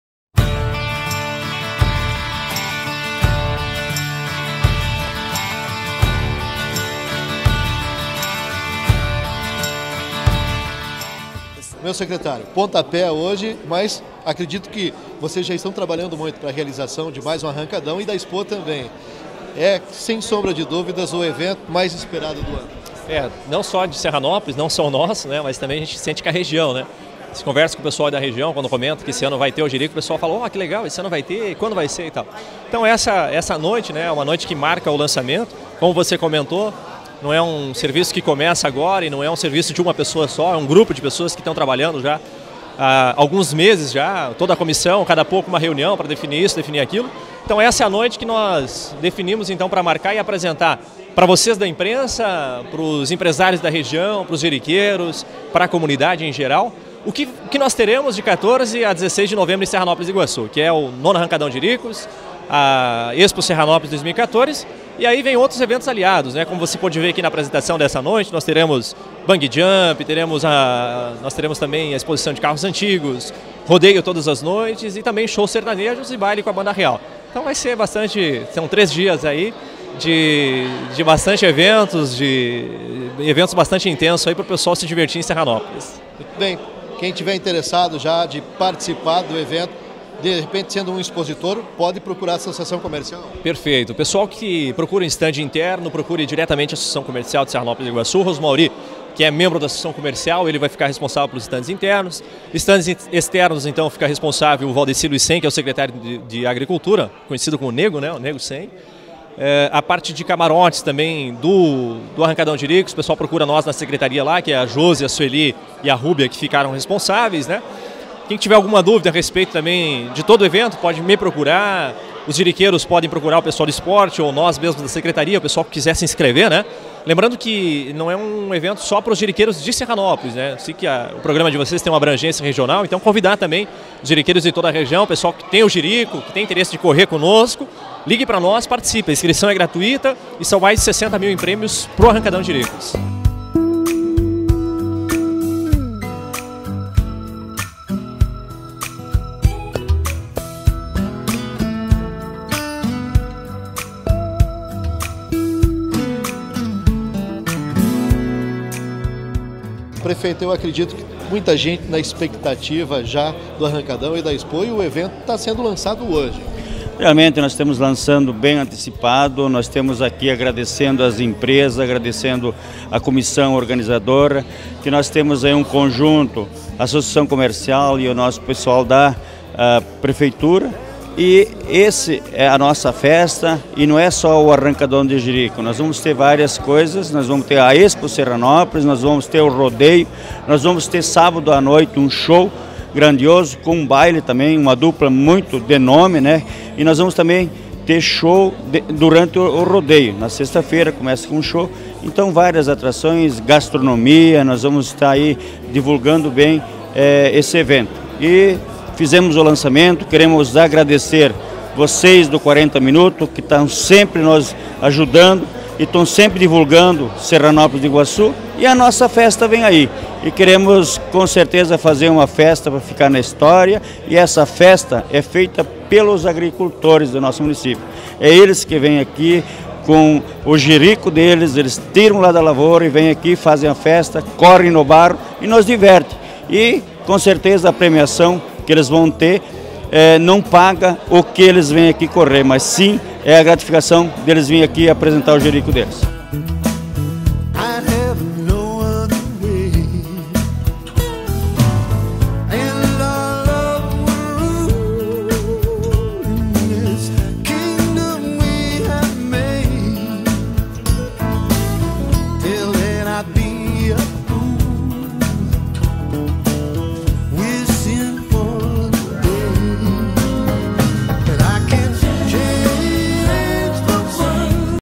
Lançamento do Arrancadão de Jericos 2014
Evento de lançamento do IX Arrancadão de Jericos 2014.